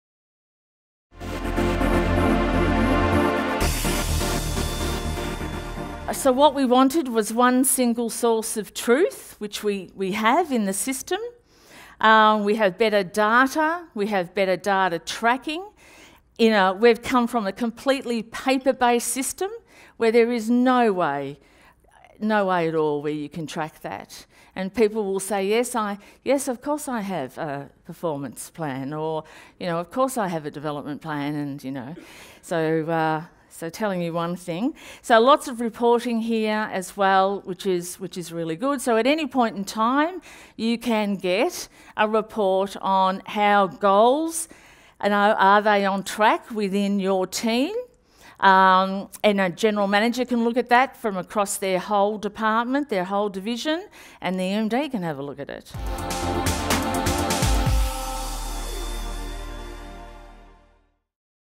Cornerstone Client Short: Melbourne Water
We're live from Converge Sydney with Linda Heron, General Manager of People & Capability at Melbourne Water. In this Cornerstone Client Short, Linda shares her insight on measuring success.